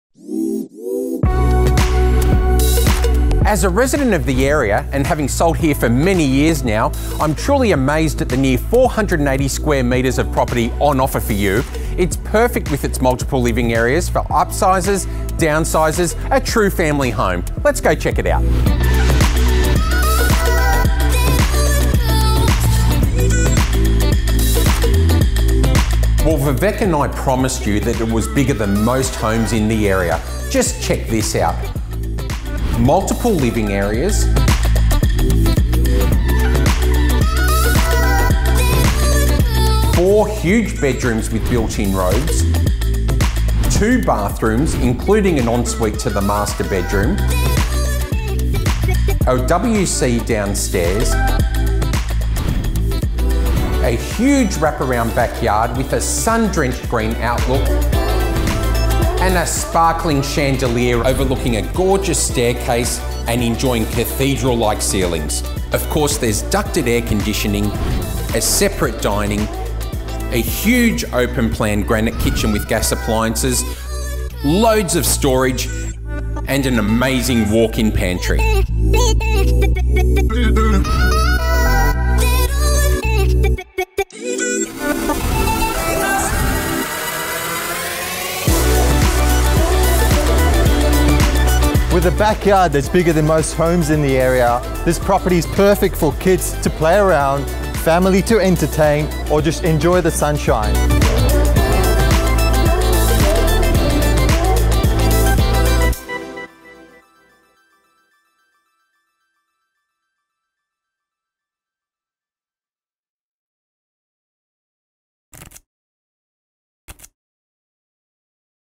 12/150 Dean Street, Strathfield South with Richard Baini & Vivek Tailor from Richard Matthews RE
12/150 Dean Street, Strathfield South with Richard Baini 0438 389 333 & Vivek Tailor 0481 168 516 from Richard Matthews Real Estate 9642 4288